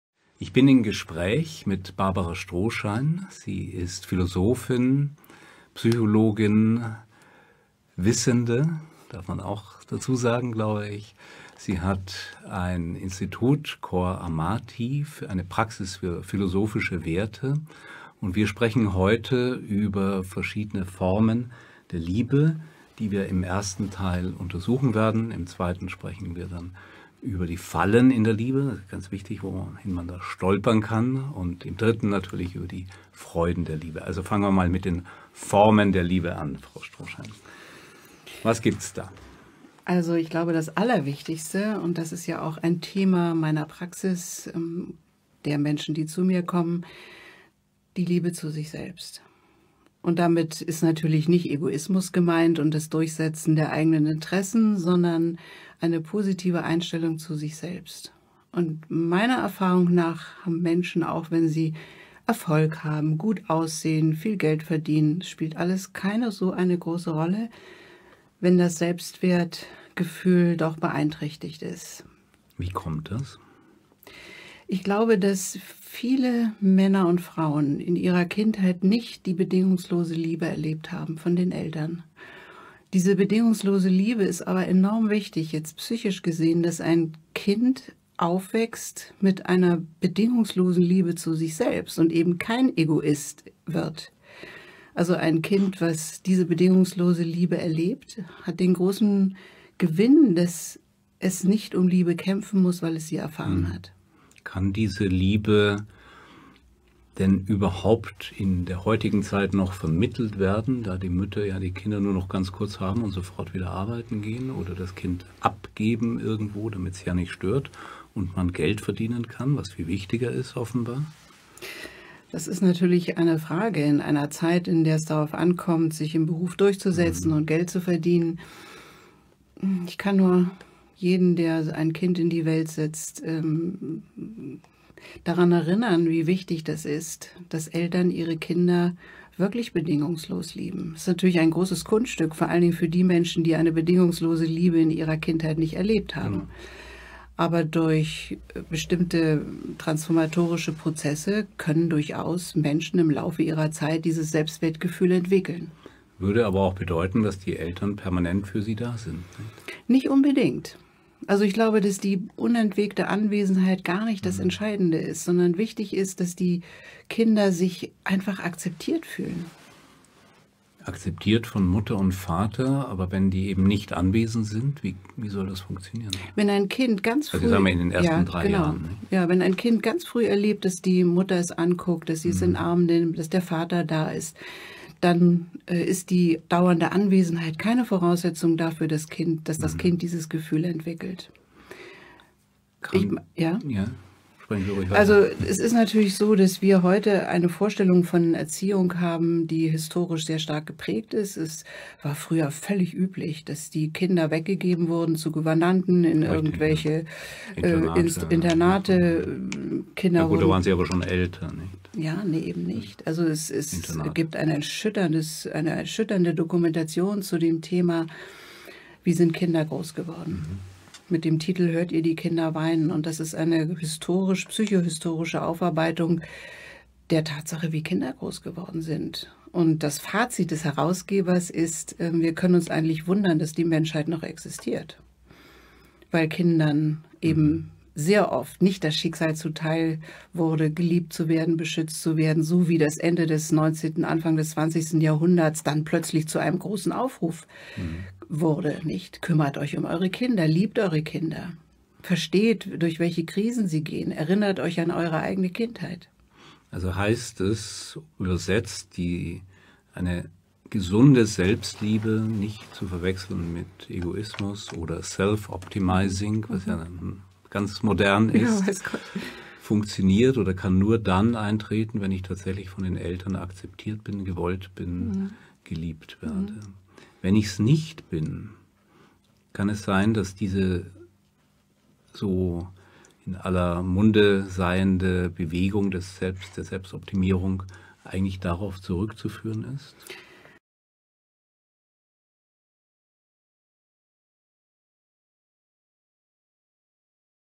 Julia and The Lovebirds: Philosophin Barbara Strohschein
Philosophin Dr. Barbara Strohschein hier im Gespräch mit dem Autor des Romans "Der Herzensheiler" Dr. Hans Christian Meiser. Über Philosophie und Psychologie & noch viel mehr...